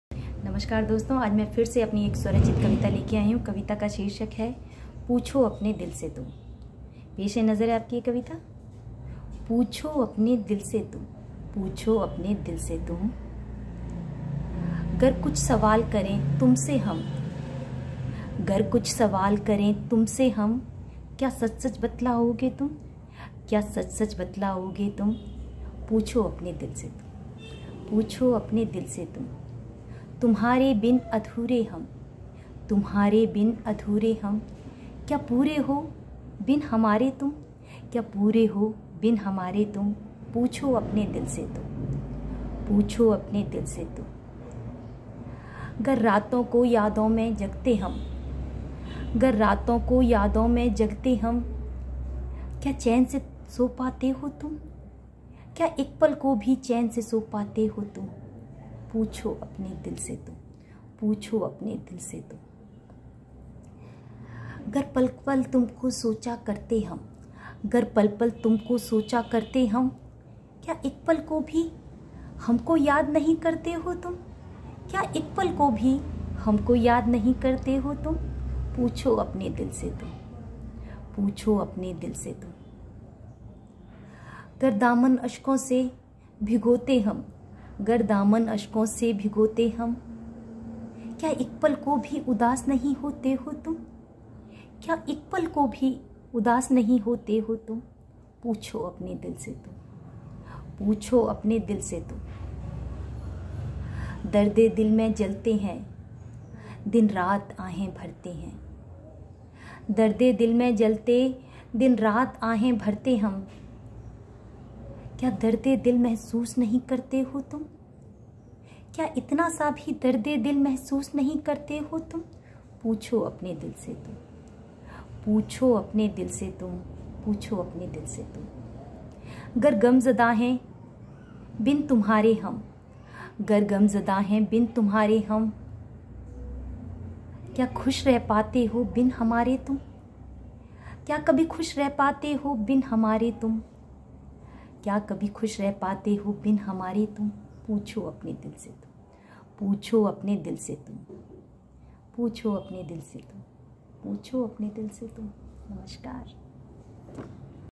"पूछो अपने दिल से तुम" एक स्वरचित कविता
नमस्कार दोस्तों आज मैं अपनी एक स्वरचित कविता लेकर आई हूं कविता का शीर्षक है पूछो अपने दिल से तुम कविता सुनने के लिए पूरा वीडियो देखें